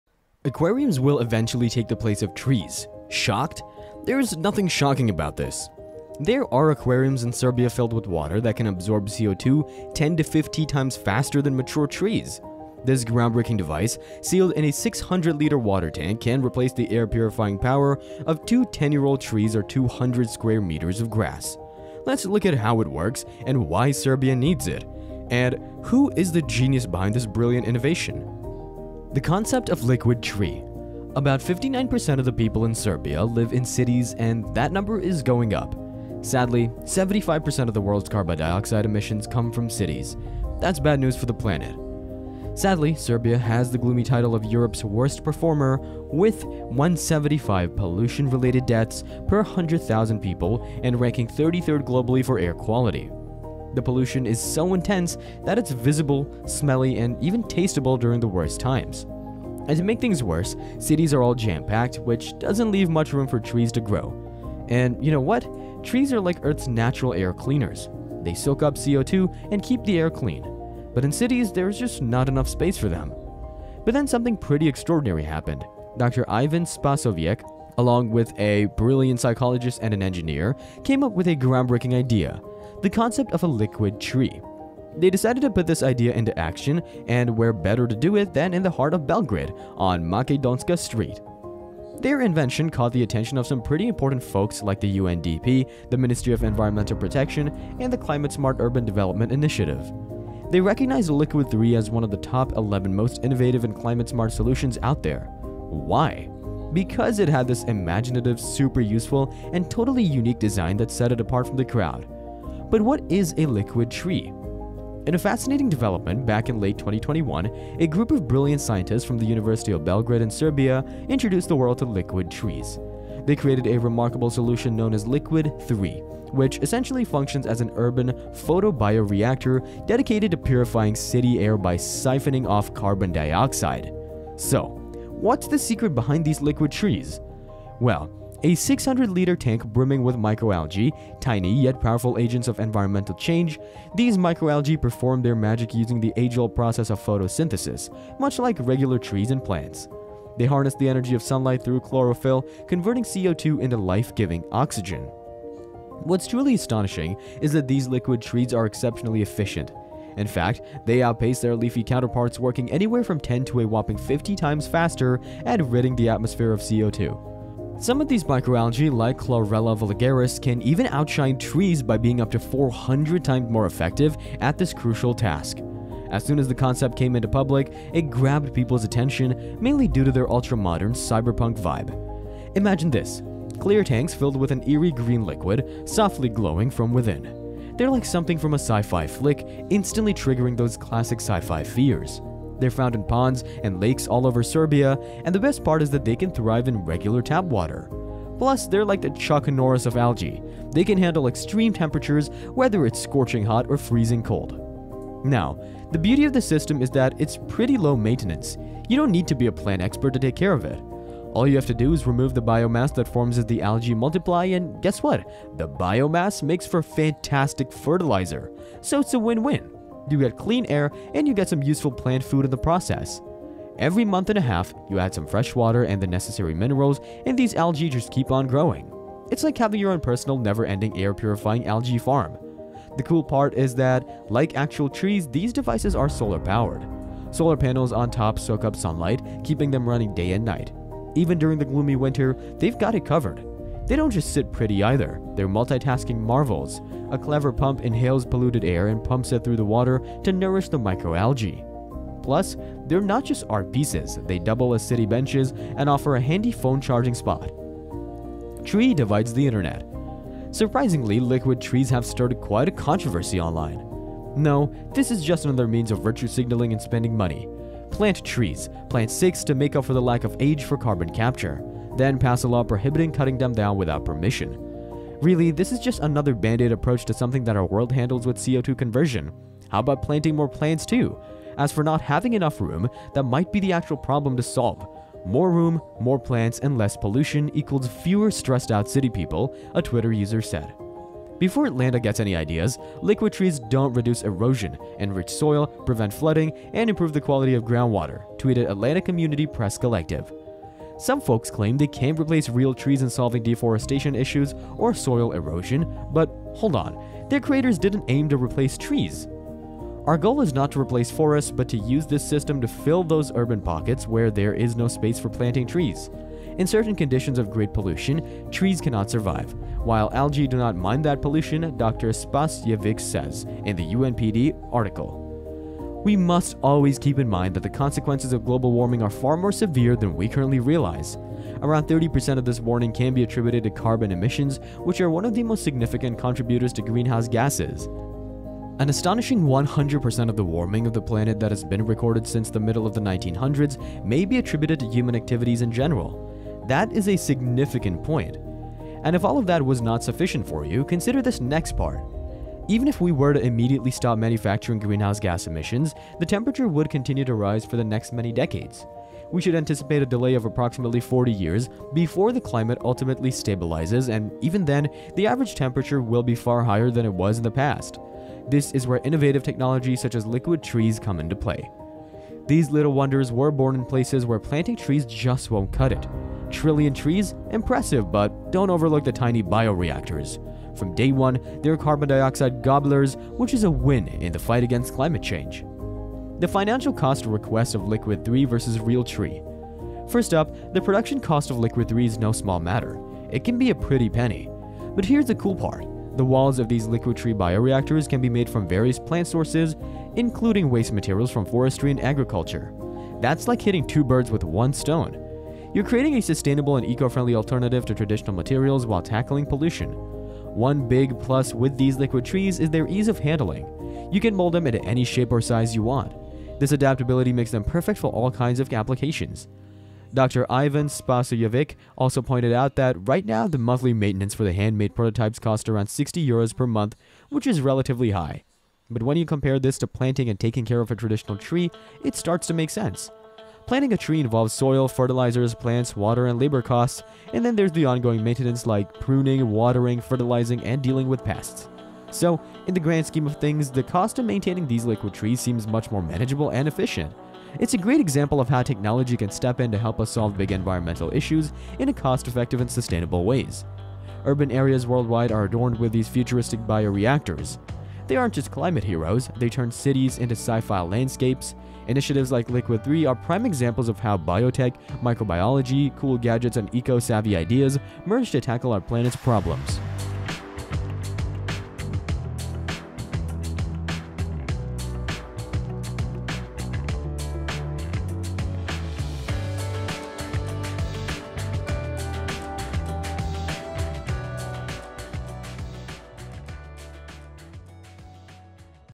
These Liquid Tress Will Save Metro Cities in FUTURE! Sustainable World
Invention of liquid tress will be one of the best invention in coming future for betterment of mankind, check how these tress are making lives better.